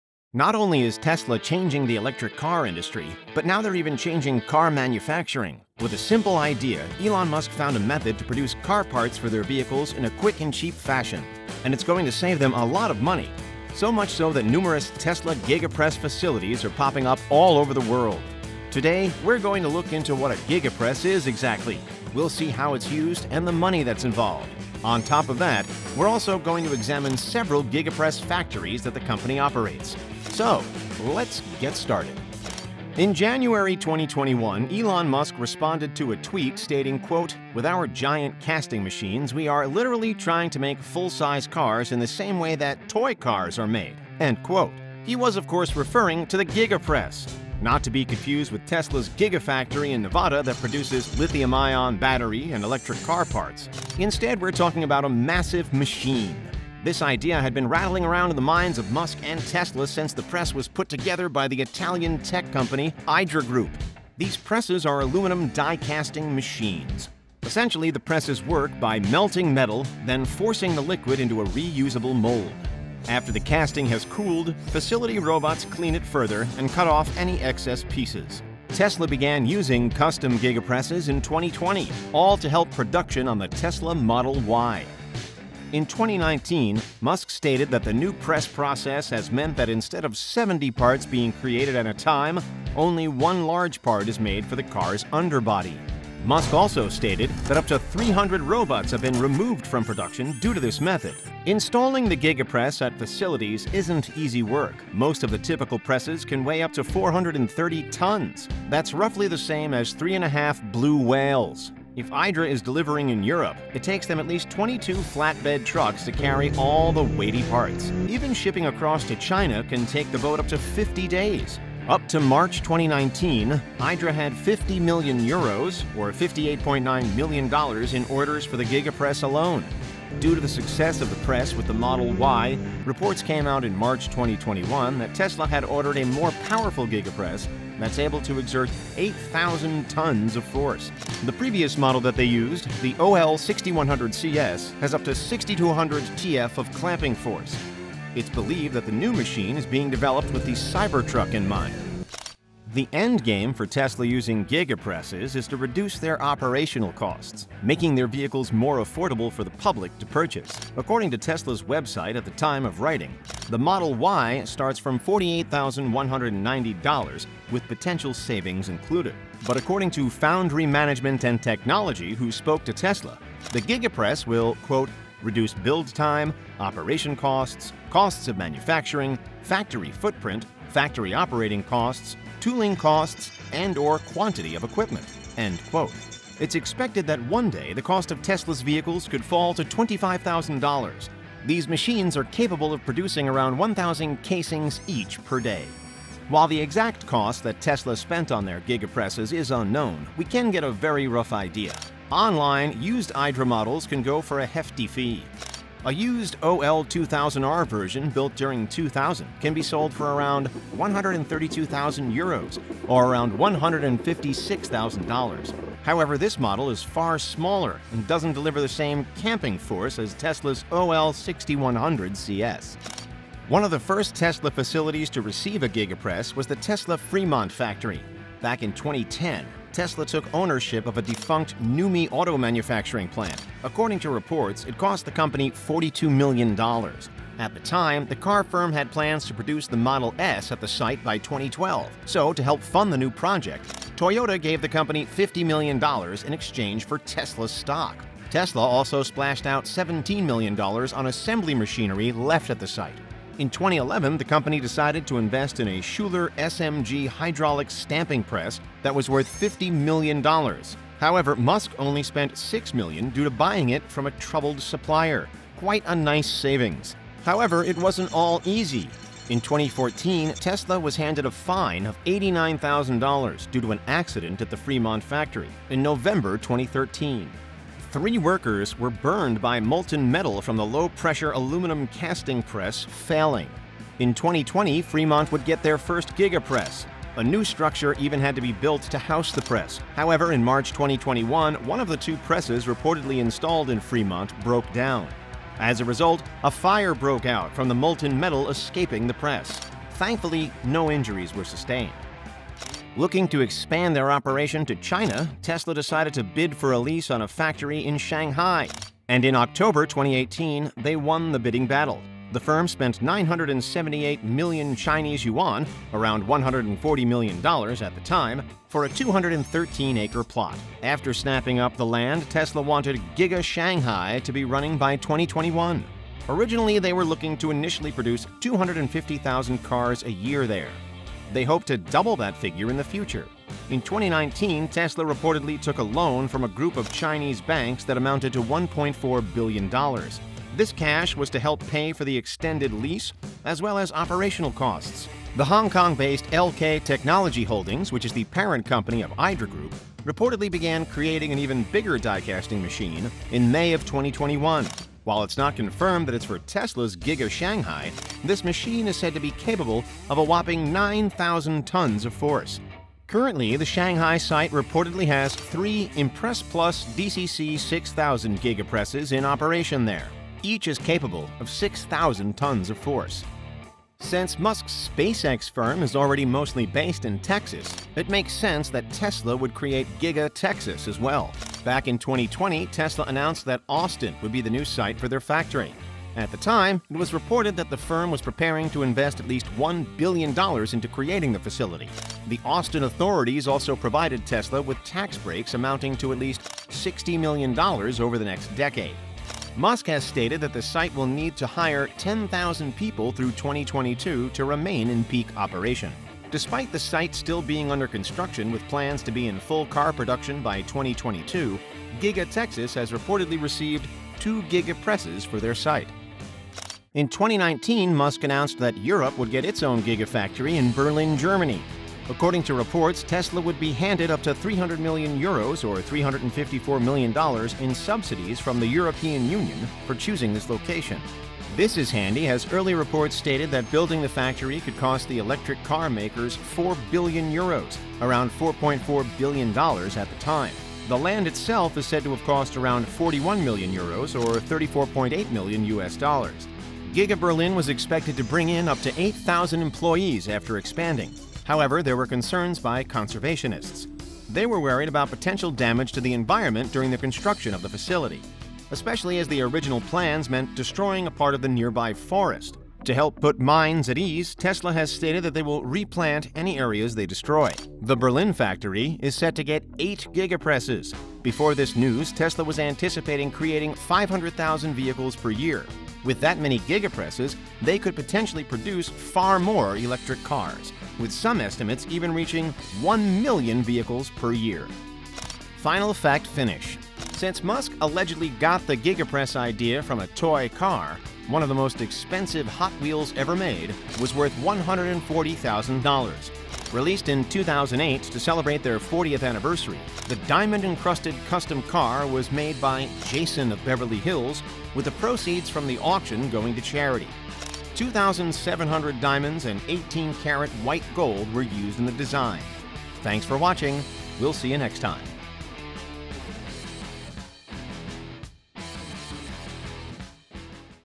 How The Giga Press Is Making Tesla More Money
Welcome back, Richest fans! Tesla regularly makes the news with their newest vehicle that will revolutionise the electric car industry. But behind the scenes, they could also be changing the car making process in general. As of now, they have the Giga Press. Inspired by how toy cars are made, this die-casting machine might save the company a fortune in operational costs. Today, we’re going to examine the Giga Press in detail. We’ll see how it works and the various costs behind it. On top of that, we’ll explore some of Tesla facilities across the globe that will have their own Giga Presses to play with.

Now for the spoiler paragraph. If you like surprises, we suggest you stop reading and watch the video multiple times. Kicking off, we have all the details on what a Giga Press is. We’ll see how it works, as well as the mammoth task of relocating one of the machines internationally. We’ll then look at the potential savings and a price estimate of how much a Giga Press might cost. We’ll then see the Tesla Fremont Factory that’s saved once saved millions on a differing casting machine. There’s Giga Shanghai that’s creating one of the most powerful Giga Presses ever! Over in Giga Texas, this facility has had a billion-dollar investment! Then there’s Giga Berlin in Germany. On top of costing a fortune, the facility is set to hold eight Giga Presses altogether! Finally, we’ll briefly look at the most expensive Hot Wheels car ever made.

Written by: Jordy McKen
Narrated by: Peter D
Edited by: Gevorg Yenoqyan

CHAPTERS:
00:00 Intro
00:33 The Giga Press
02:33 Money Saved
03:44 Tesla Fremont Factory
05:03 Tesla Giga Shanghai
06:14 Giga Texas
06:57 Giga Berlin